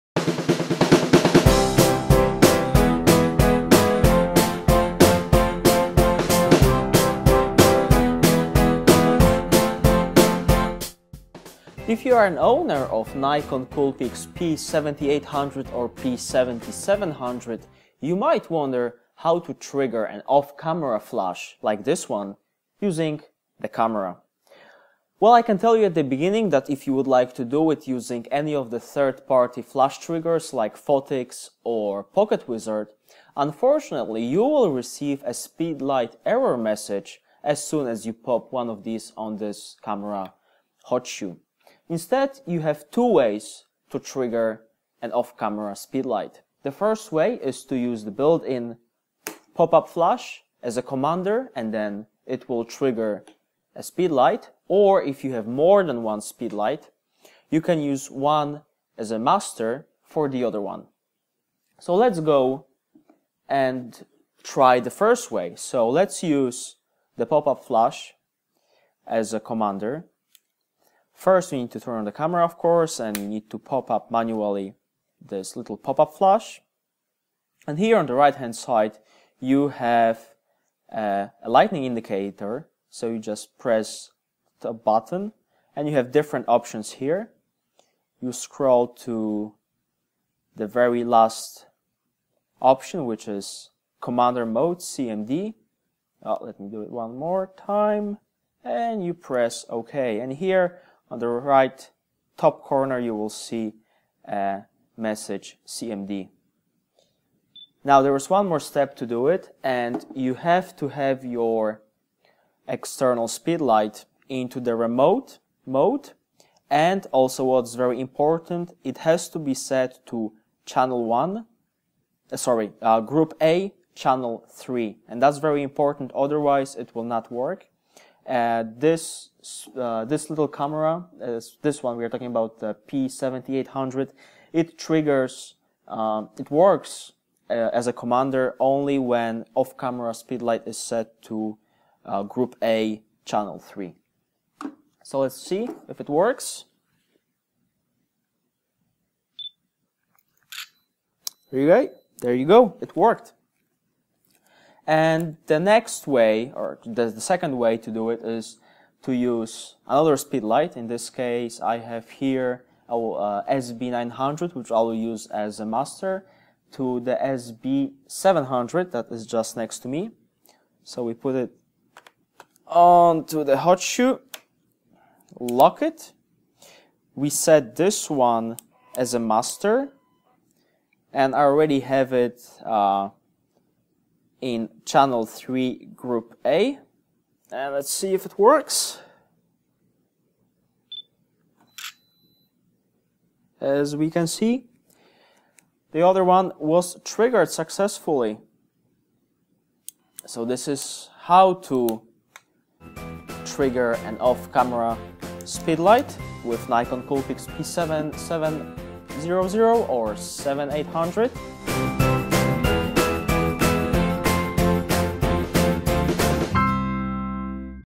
Nikon Coolpix P7800 - How to trigger off-camera flash or speedlight
Although Nikon Coolpix P7800 has a fully functional hot-shoe (at least that what Nikon claims it to be), triggering off-camera speedlights and flashes with P7800 is quite tricky. In this video I show you the two ways for triggering off-camera speedlights with Coolpix P7800.